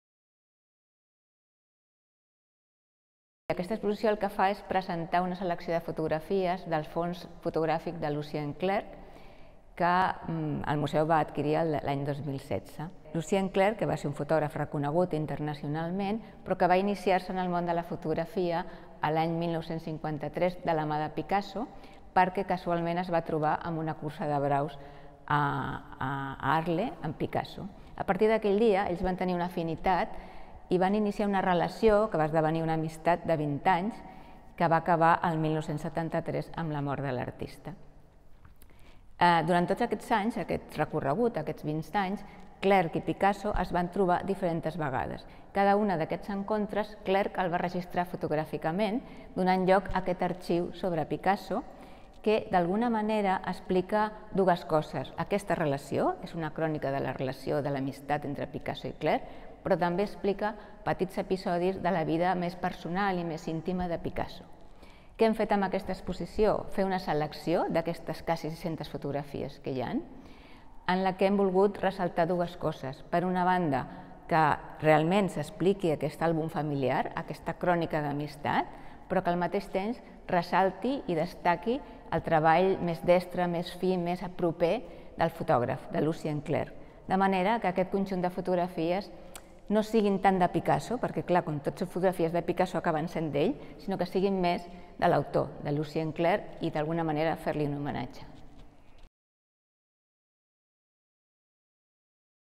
Sílvia Domènech, comissària de Picasso-Clergue ens explica l'exposició.
PICASSO – CLERGUE

Del 22 de juny de 2022 al 23 d'octubre de 2022

«Picasso-Clergue» presenta en forma de crònica una selecció del fons fotogràfic Lucien Clergue, que va adquirir l’any 2016 el Museu Picasso, relatiu a Picasso: la història en imatges que Clergue va fer de Picasso al llarg de les nombroses trobades que van tenir lloc al sud de França, on tots dos vivien. «Picasso-Clergue» és alhora un àlbum de família i el diari d’una amistat.